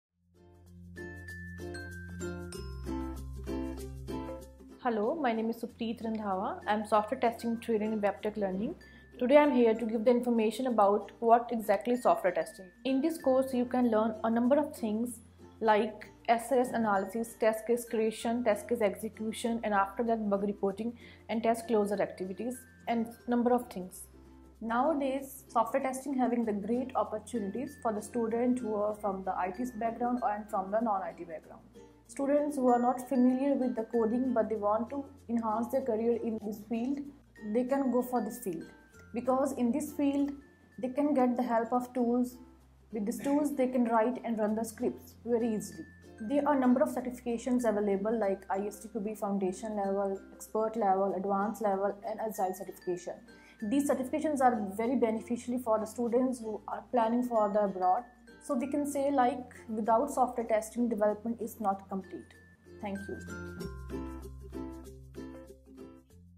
Certified Software Testing Training at WebtechLearning- Web education academy
Know about Software testing Training & its Future Scope in India as well as Overseas. Meet Ms. Supreet Randhawa-Software Testing Professional at WebtechLearning-academy in Chandigarh.
WebtechLearning - A Web Education ISO 9001:2000 Certified and Training Academy in Chandigarh, India. Training by Industry experts.